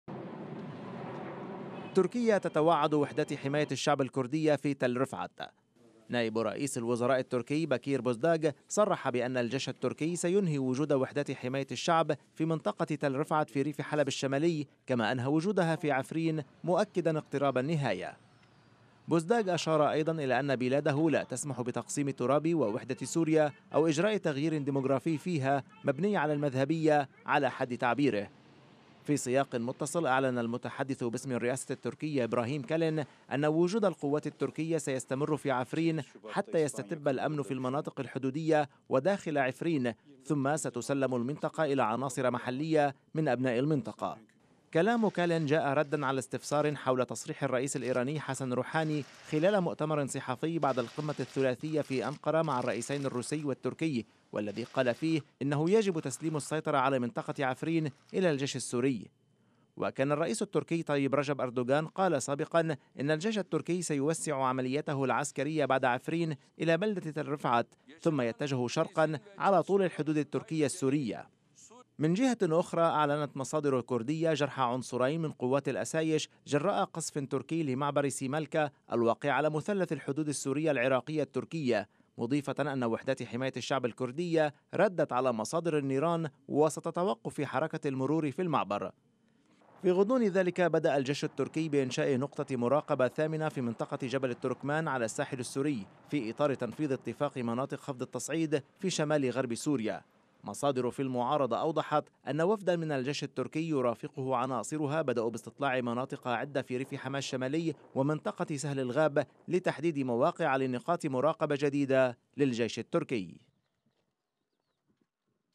تركيا تهدد وحدات حماية الشعب الكردي في تل رفعت
قال نائب رئيس الوزراء التركي إن الجيش التركي سينهي وجود وحدات حماية الشعب الكردي في منطقة تل رفعت على غرار عفرين